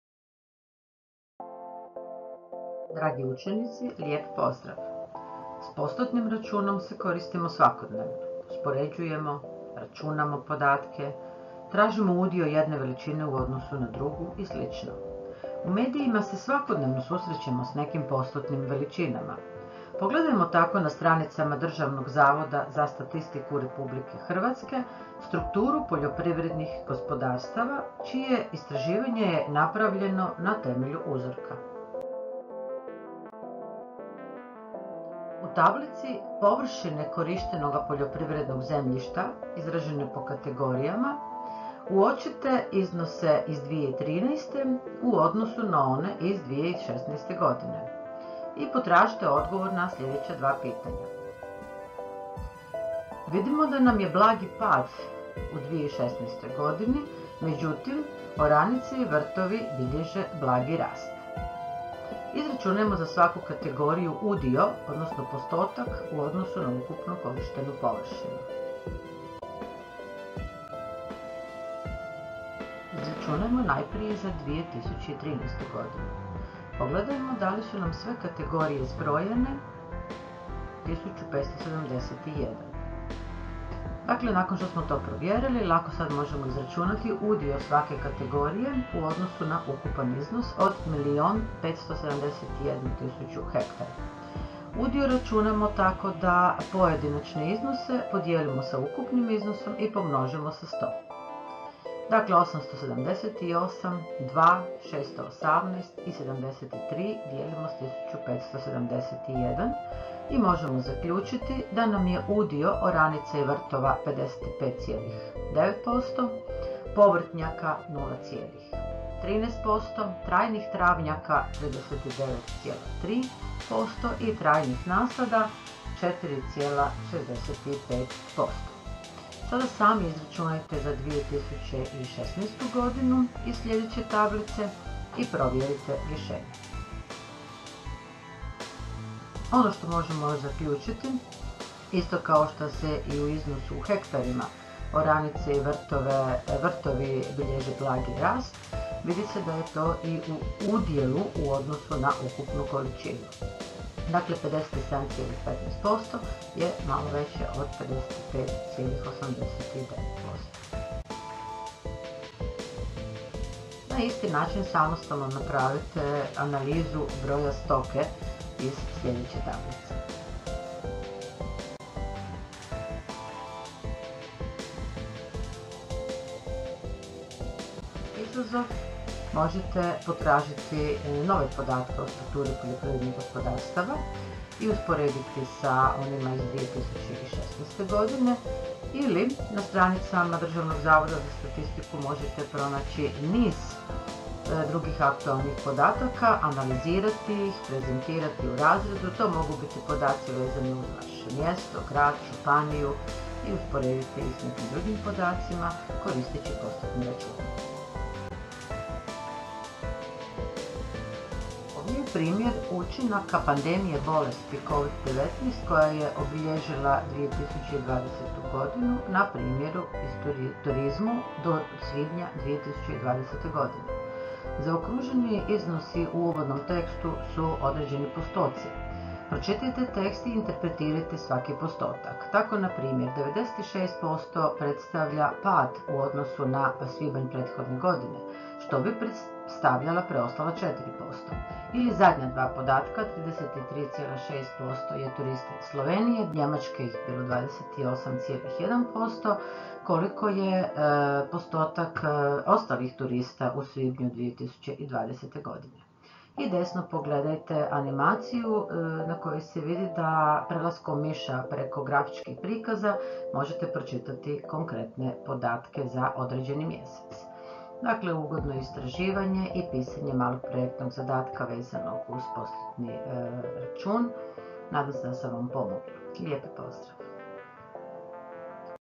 Primjena postotnog računa
U ovom videu možete ponoviti postotni račun te kako izračunati udio nekog podatka u ukupnom iznosu. Koriste se podatci sa stranica DZS-a
Još neke poveznice na kojima možete pronaći podatke za dodatnu analizu i primjenu postotnog računa: